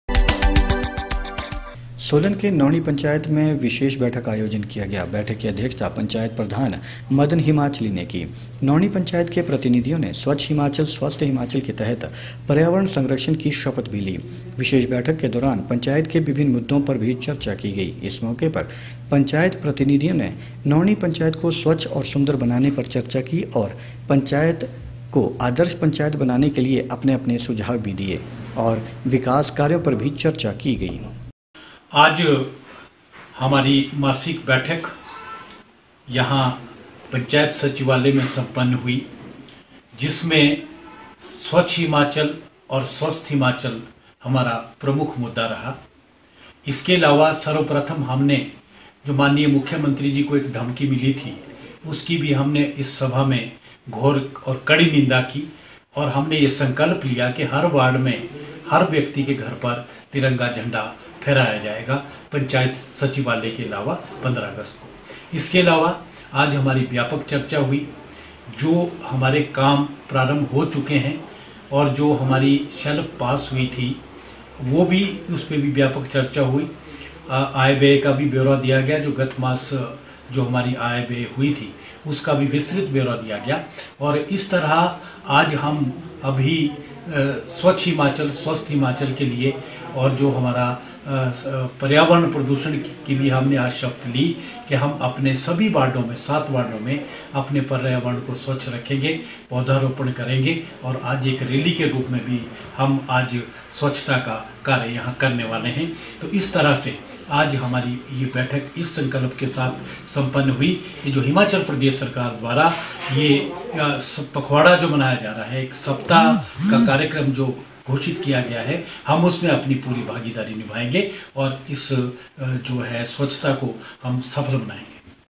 solan नौणी पंचायत के सभी घरों में फहराया जाएगा राष्ट्रीय ध्वज : मदन हिमाचली
सोलन की नौणी पंचायत  में  विशेष बैठक का आयोजन किया गया।  बैठक की अध्यक्षता पंचायत प्रधान मदन हिमाचली ने की। नौणी पंचायत  के प्रतिनिधियों ने   स्वच्छ हिमाचल   स्वस्थ हिमाचल के  तहत पर्यावरण संरक्षण की शपथ   भी ली  ।  विशेष बैठक के दौरान , पंचायत के विभिन्न मुद्दों पर, चर्चा की गई | वही इस मौके पर  पंचायत प्रतिनिधियों ने नौणी पंचायत को स्वच्छ और सुंदर बनाने के लिए  चर्चा की।  और नौणी पंचायत को आदर्श पंचायत बनाने के लिए अपने अपने कीमती सुझाव भी दिए।  वहीँ पंचायत के विकास कार्यों को लेकर भी चर्चा की गई।